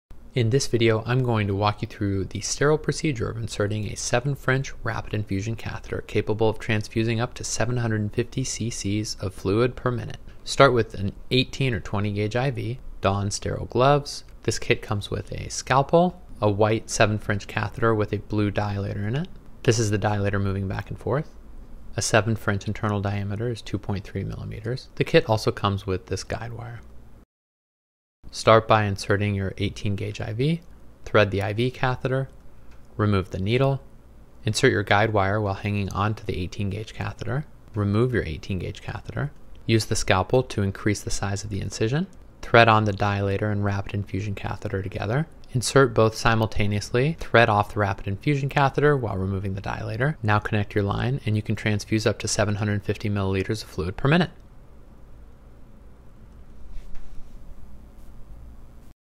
Insertion technique for huge IV - the Rapid Infusion Catheter
See how one of the biggest IVs in anesthesiology is inserted. This is a 7 French (2.3mm) rapid infusion catheter capable of transfusing fluid at rates of 750 mL/min when connected to a Belmont rapid infuser.

Thanks to Dr. Adam Levine, program director of anesthesiology at Mount Sinai Hospital, New York City, for allowing me to use the equipment in this video!